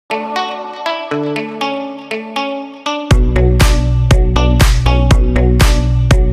Led torch into beautiful bed lamp
Beautiful bed lamp
how to make green glowing bed lamp
torch light and himalaya moisturising cream into beautiful bed lamp